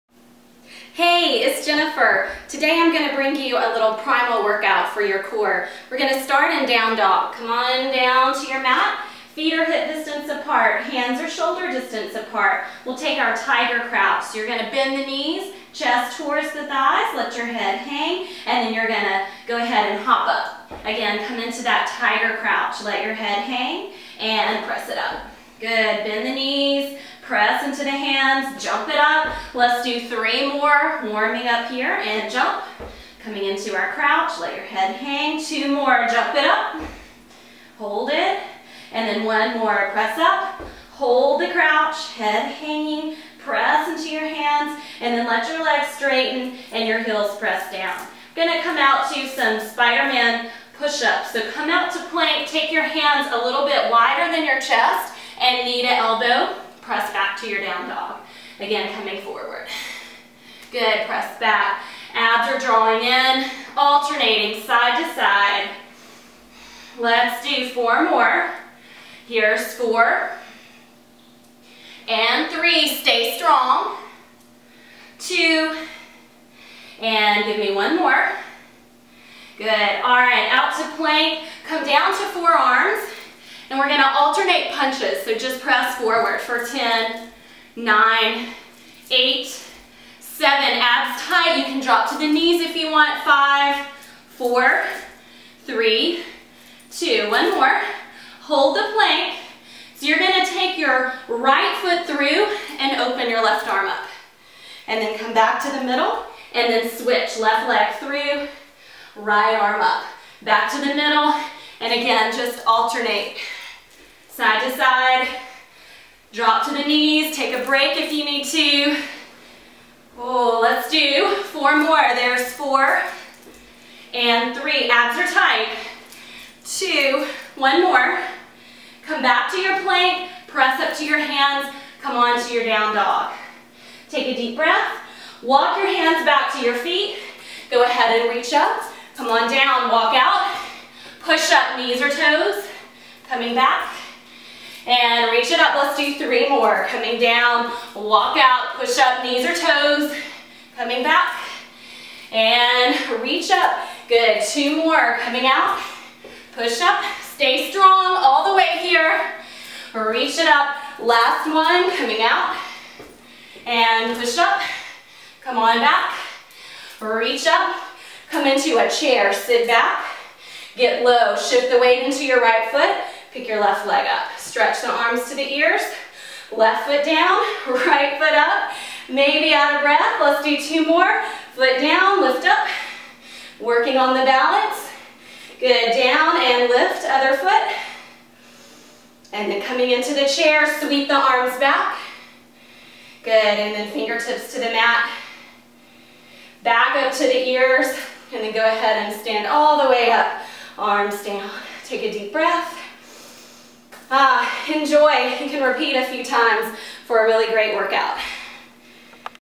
Primal Core Workout
Get on the ground with your primal instincts for a fun core workout!